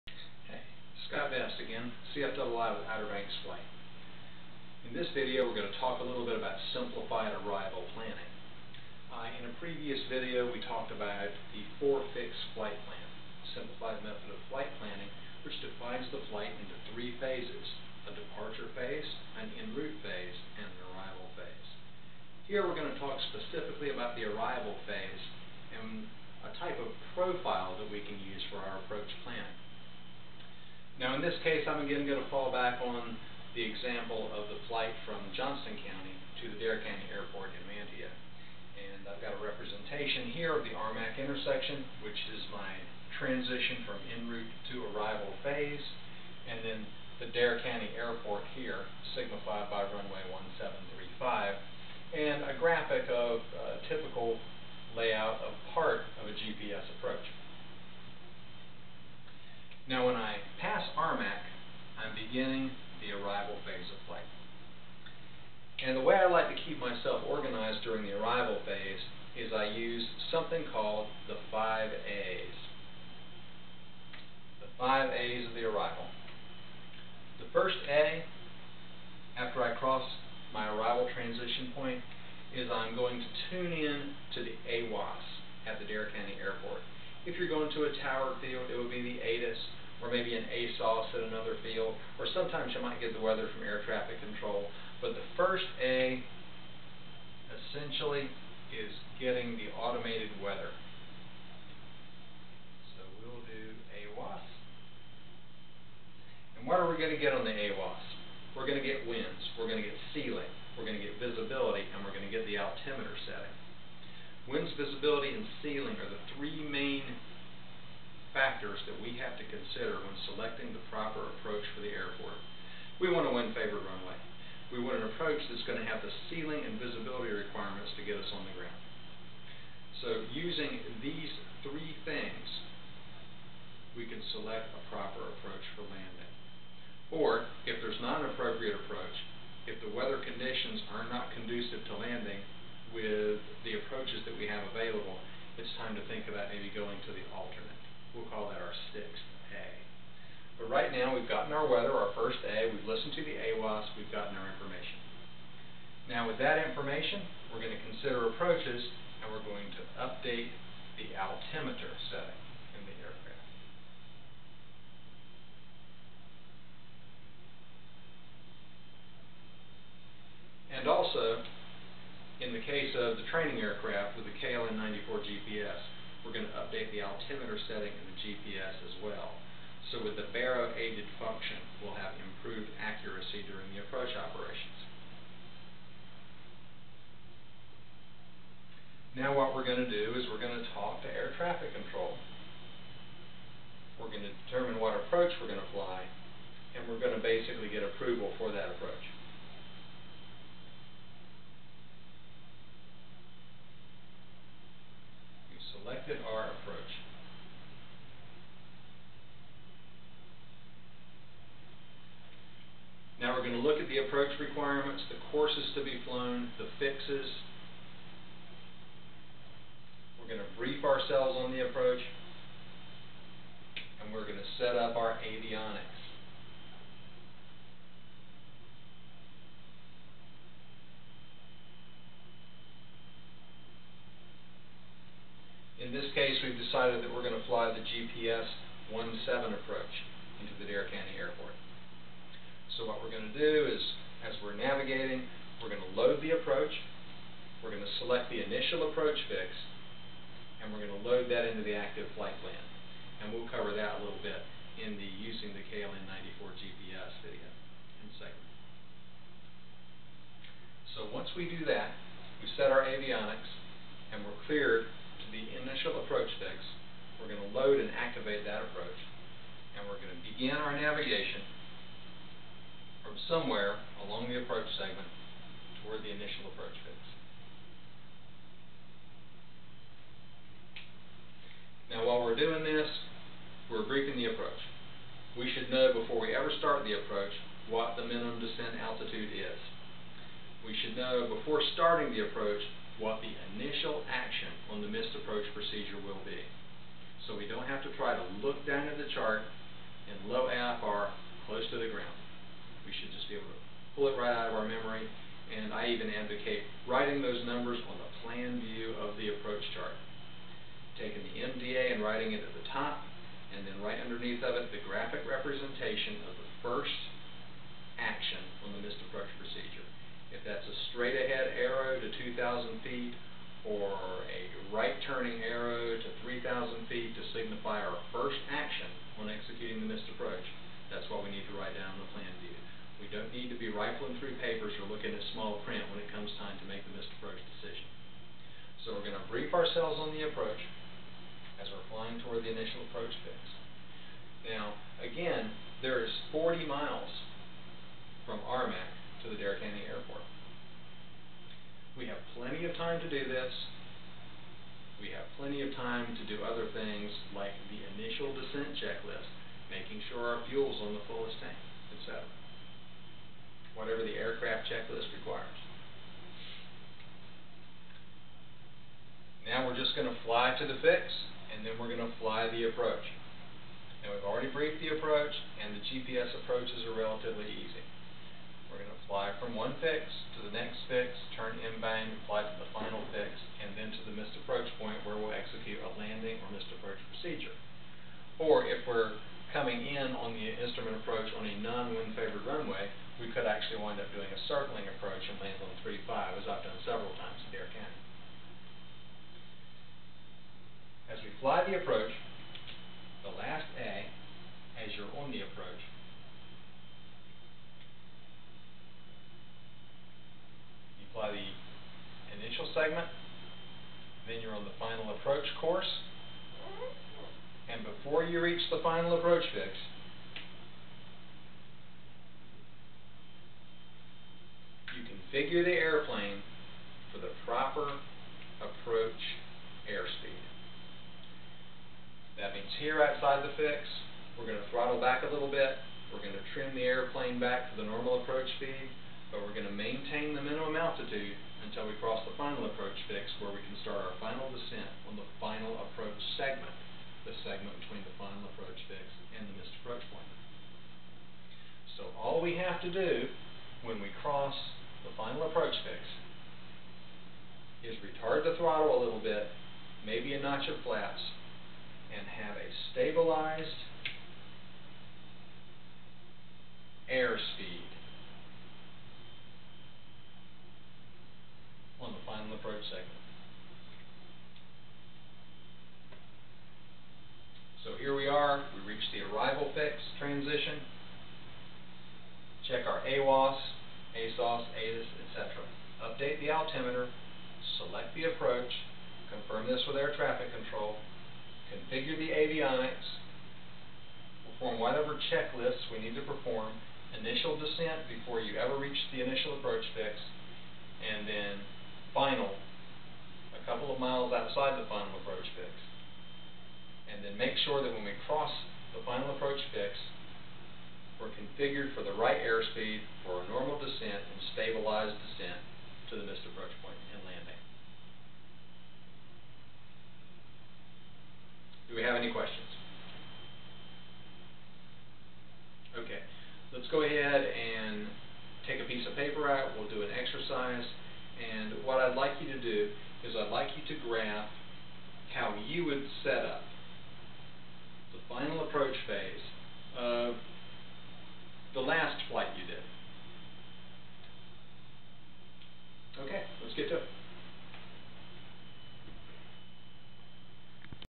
OBX FLIGHT: Planning the Arrival
Scott Best, CFII explains the 5 A's of the arrival phase of the flight plan. The 5 A's organizes the pilot tasks in a manner to reduce workload in the terminal or arrival phase of flight.